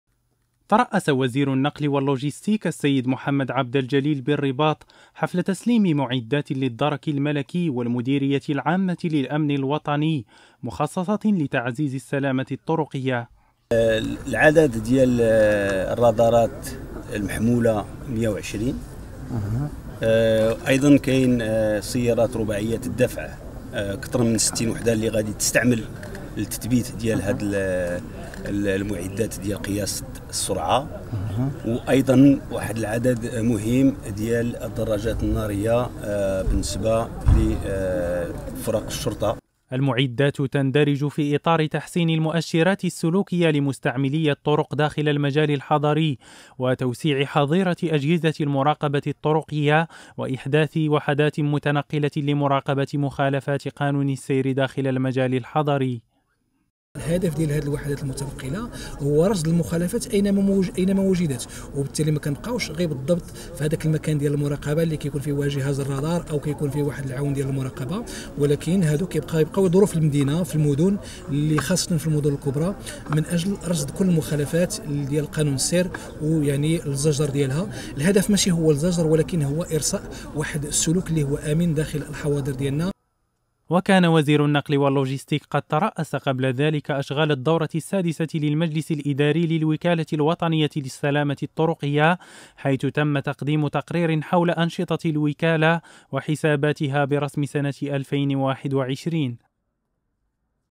سلامة طرقية: تسليم معدات للدرك الملكي والمديرية العامة للأمن الوطني
WebTV  officiel du Ministère du Transport et de la Logistique